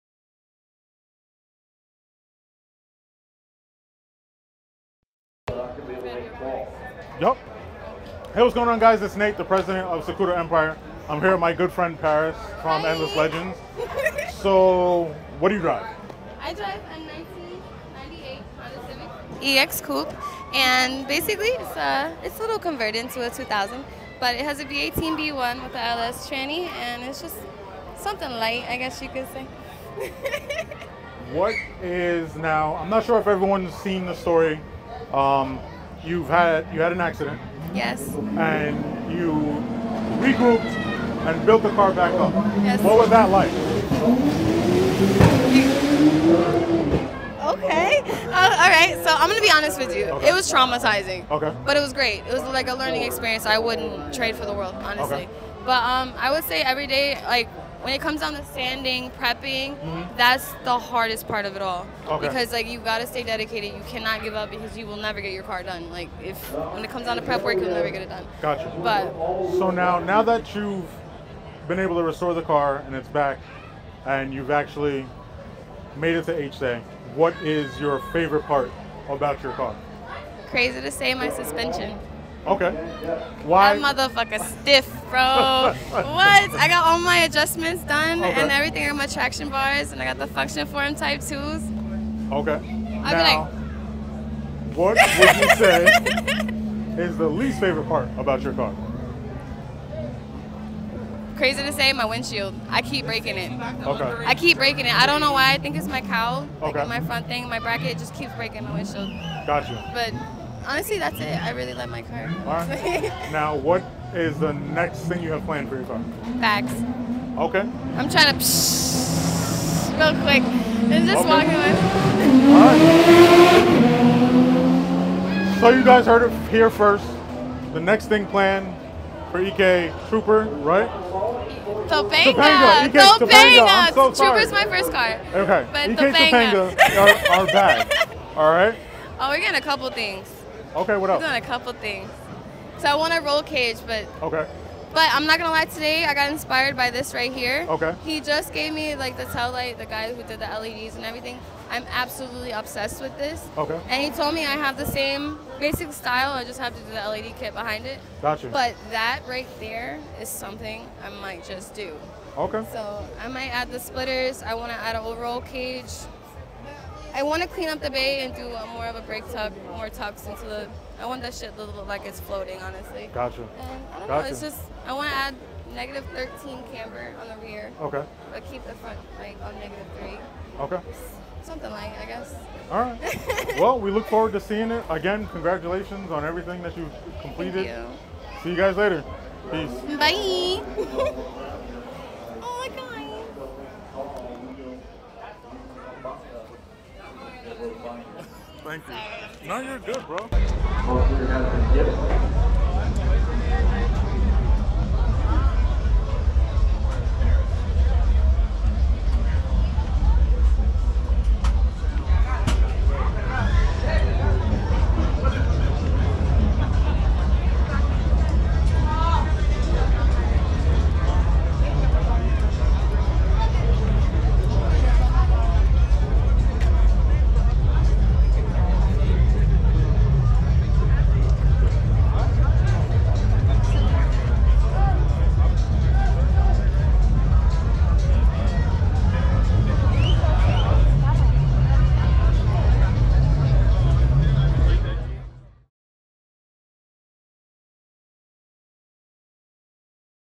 Check out EK Topanga
At HDAY we caught up with Paris and EK Topanga.  Check it out.